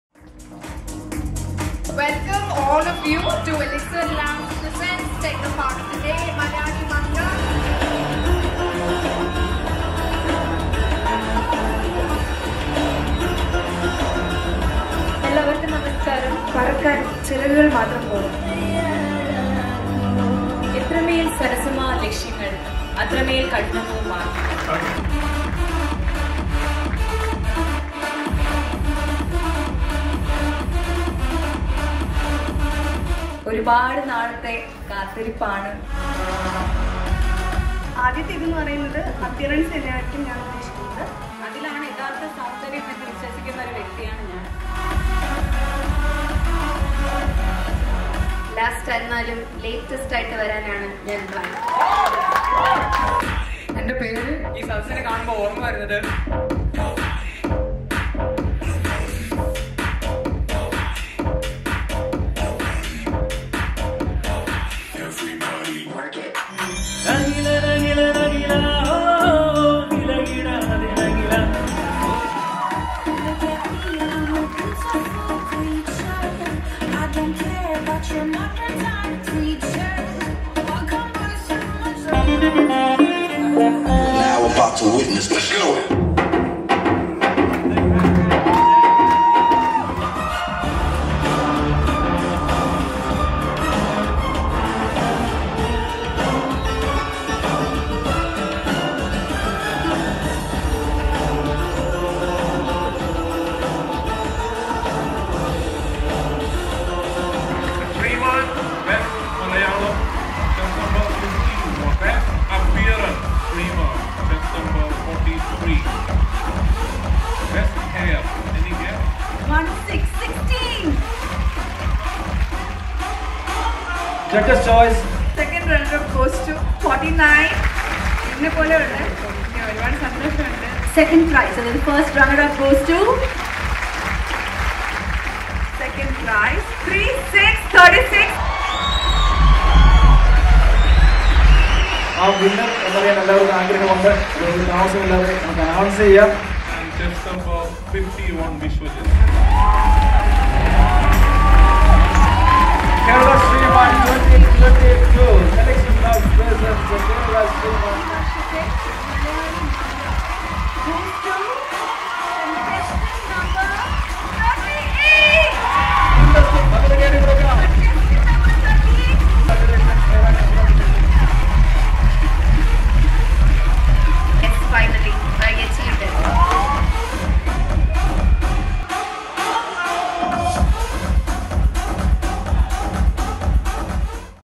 Elixr Labs presents TechnoparkToday’s Malayali Manka and Kerala Sreeman 2022
On Sunday, 4th December 2022, Technopark's glamorous crowd walked the ramp with confidence and attitude to prove they're more than just brains at the grand finale of TechnoparkToday’s Malayali Manka and Kerala Sreeman 2022 presented by Elixr Labs. The event held at C-DAC Auditorium, Technopark was also supported by Xilligence, Reflections, SEQATO and Deeshna.

Here's a sneak peek.